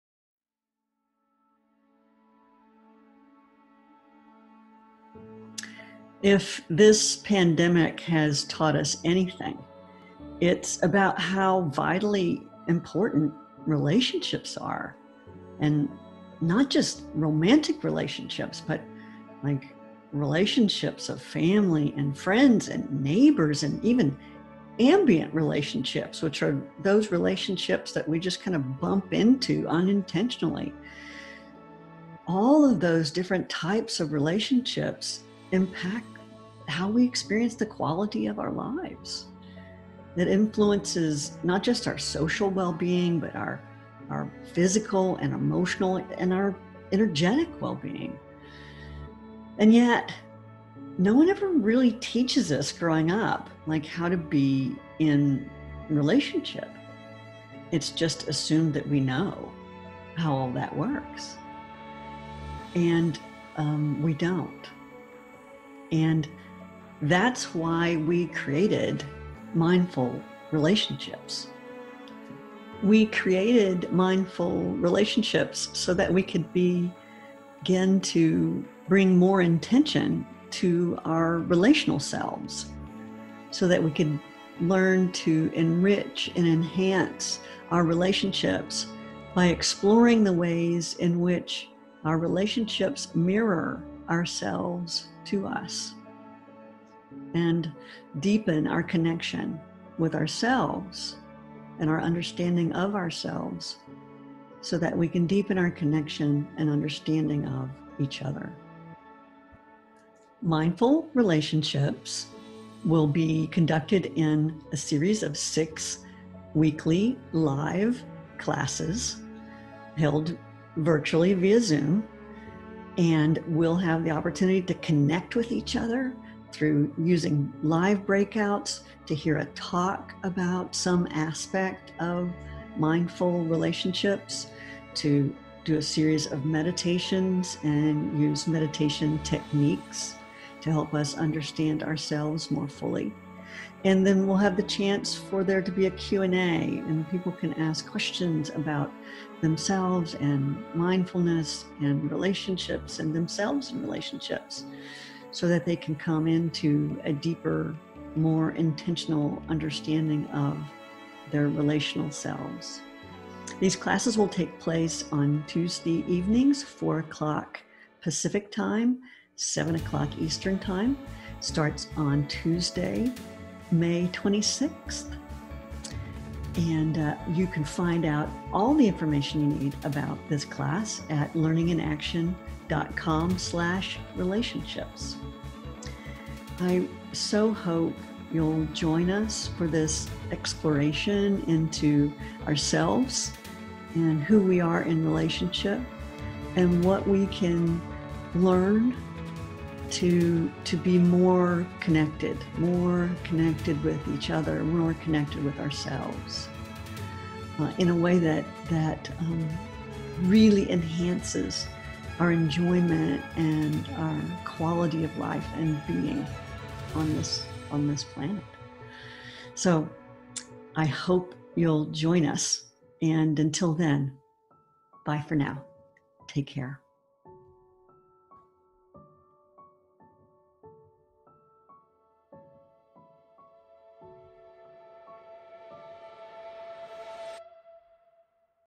How to Be in Wise Relationship with Self, Others and the World | Mindful Relationships
Mindful Relationships is a 6-week program designed to enhance your connection with yourself, your loved ones, and even the challenging ones in your life. This live, virtual course will include a talk, meditation and Q&A and is based upon the foundation of mindfulness and mindfulness meditation practices.

Through this program, you'll gain an understanding of how our relationship with ourselves impacts our relationships with others. We’ll explore methods to cultivate a practice of presence and acceptance, and you’ll learn the skillful means you need to engage with even the most difficult relationships.

Our relationships are perhaps the single most important aspect of our lives. Our relationships with friends, family, neighbors, co-workers, and importantly ourselves directly affect the quality of our lives. This course will provide both inspiration and practical methodology for using meditation techniques to begin healing the relational divides within ourselves and others.